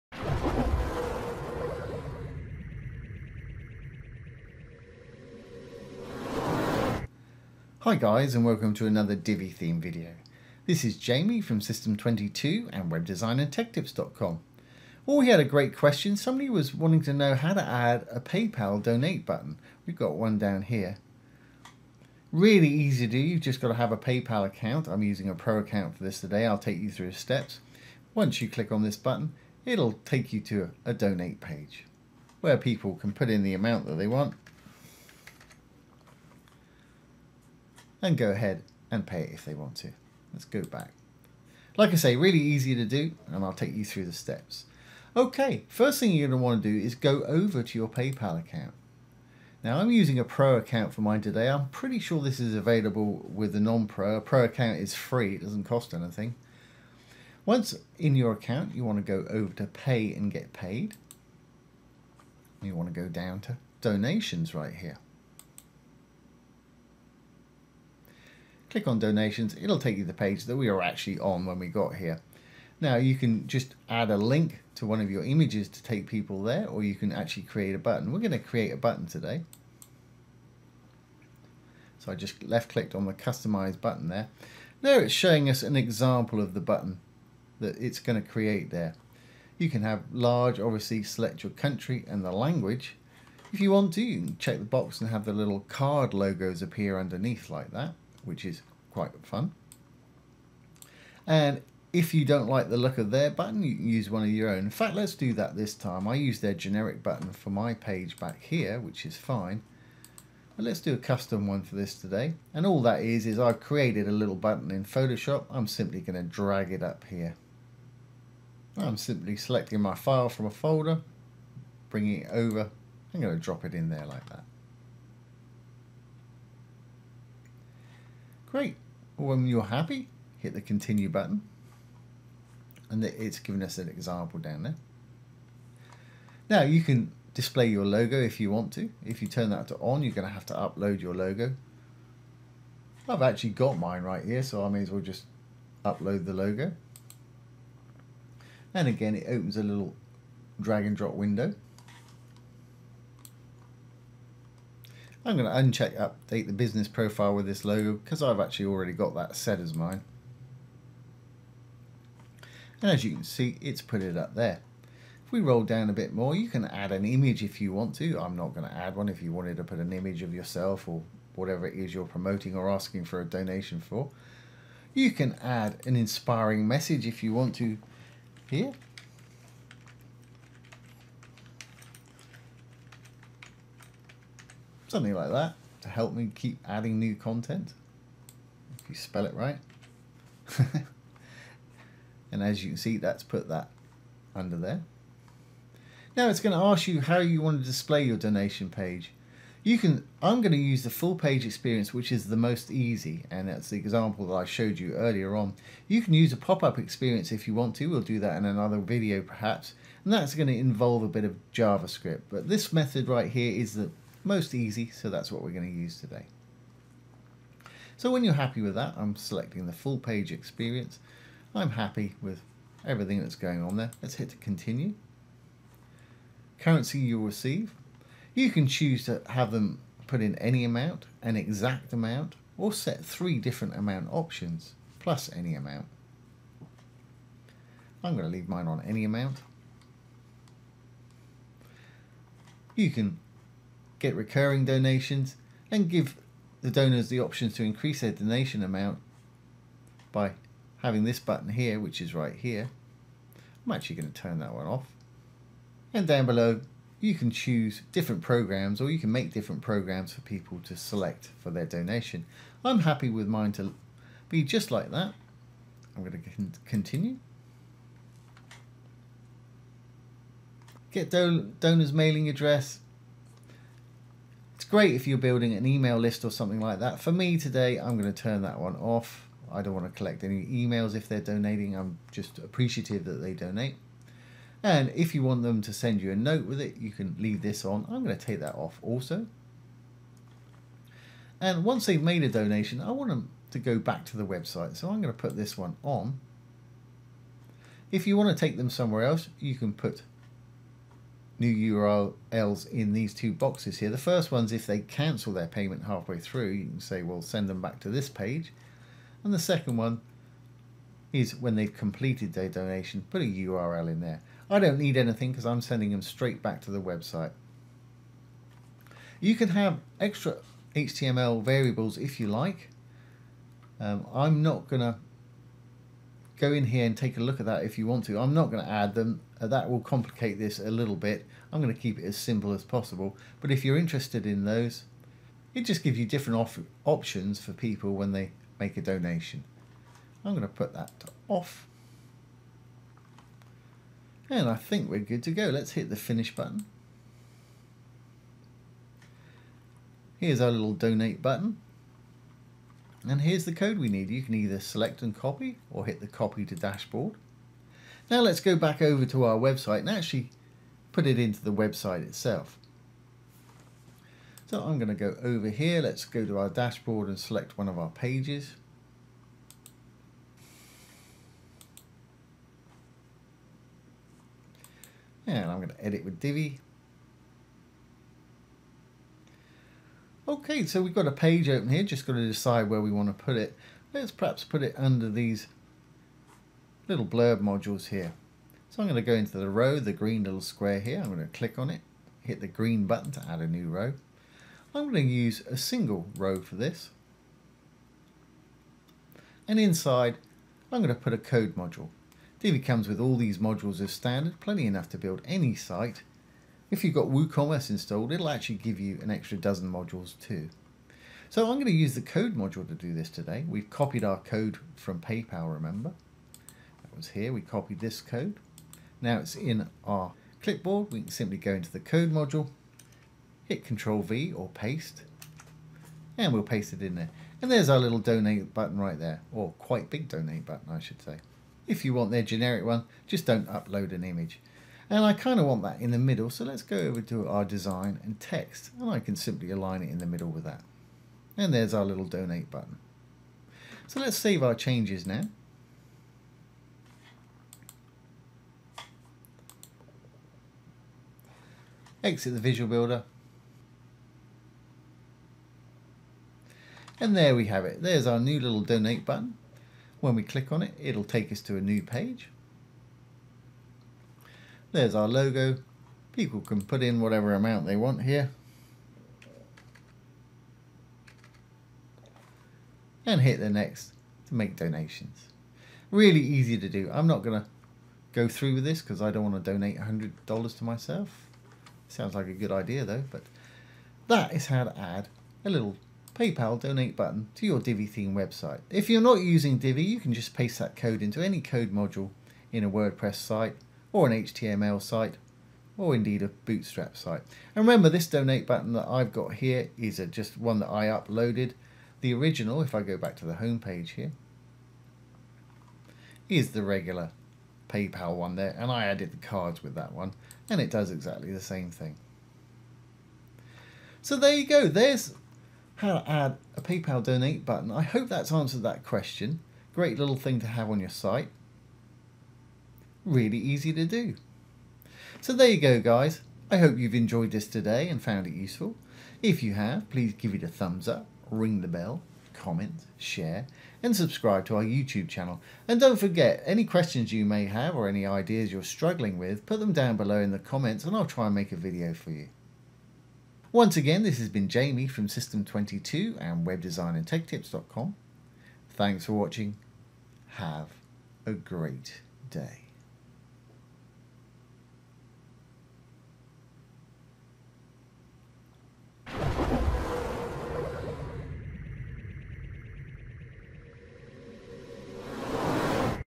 How To Add A Paypal DONATE Button To Your Website 👍👈
How to add a paypal donate button with the Divi Theme. Today we will be demonstrating how to use a Divi Code Module. We will be going over to paypal to create the donation button.Once created we can copy the HTML code and paste it into out Divi Code Module. This is a very useful, and a great feature to have on your site.

We will cover:
Where to go on paypal to create the donation button.
Styling and custom features.
Button options after donation.
Copying the HTML code.
Creating a space on the website for the donate button.
Adding a code module and inserting the code.

We are going to be using the Divi theme to create some great effects in this series of videos. The Divi theme has some great modules and effects. With a little work you can achieve some great eye-catching effects to enhance the look and user experience of your website.
This is a great feature to have on your website, and very easy to do. There is a tiny bit of coding involved today to build this feature. But, it is all generated for you, and all you need to do is copy and paste the code into your site. Everything else is being done with the Divi Themes incluided features.
So, follow along with the video and see how to add a paypal donate button using the Divi Theme.

v=ZAO2MH0dQtk&list=PLqabIl8dx2wo8rcs-fkk5tnBDyHthjiLw

v=rNhjGUsnC3E&list=PLqabIl8dx2wq6ySkW_gPjiPrufojD4la9

CHAPTERS

00:00 Introduction
00:48 Where to go on paypal to create the donation button
01:22 Styling and custom features
01:53 Adding a custom button
02:15 Add your logo
02:56 Insert a custom message
04:57 Button options after donation
05:50 Copying the HTML code
06:11 Creating a space on the website for the donate button
06:39 Adding a code module and inserting the code
07:32 Result